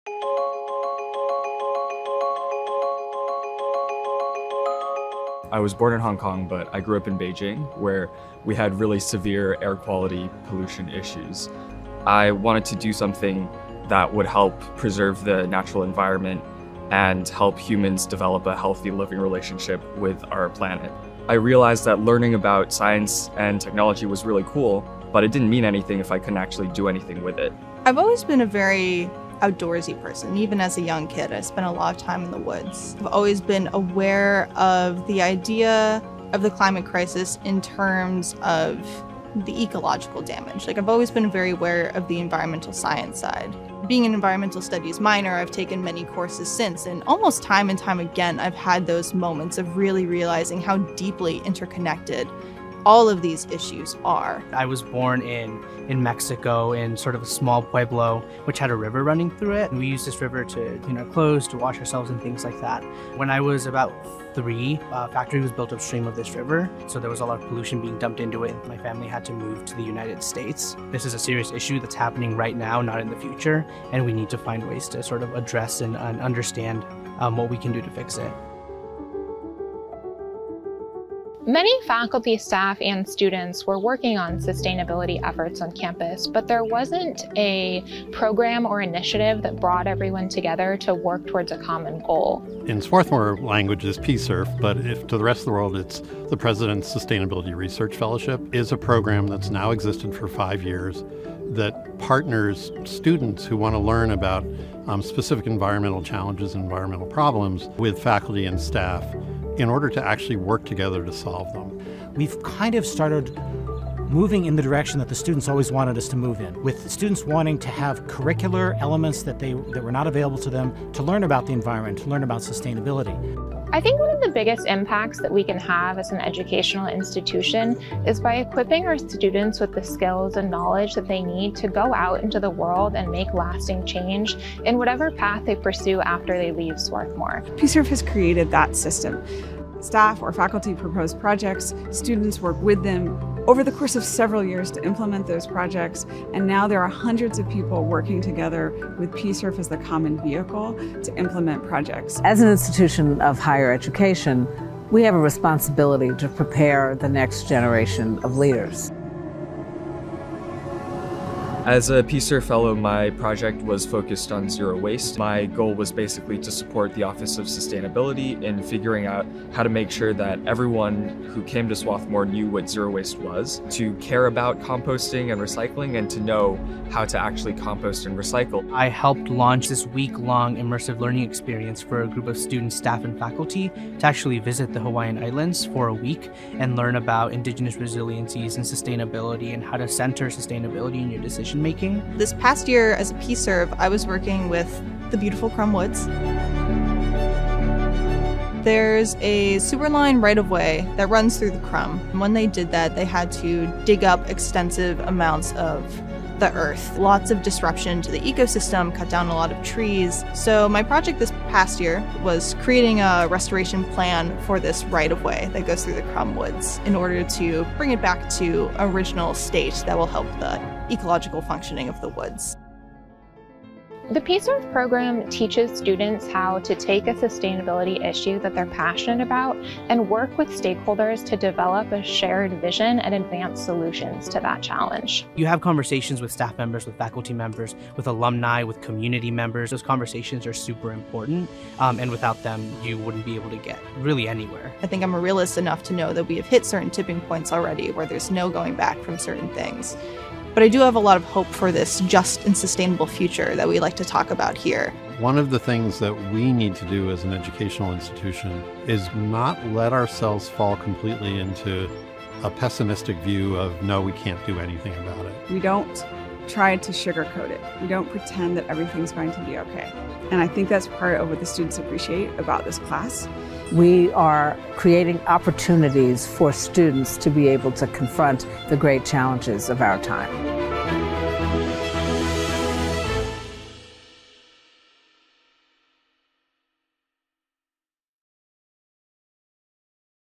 President's Sustainability Research Fellowship
The President’s Office, the Office of Sustainability, the Environmental Studies Program, and the Lang Center for Civic and Social Responsibility jointly host the high-impact learning program, The President’s Sustainability Research Fellowship (PSRF).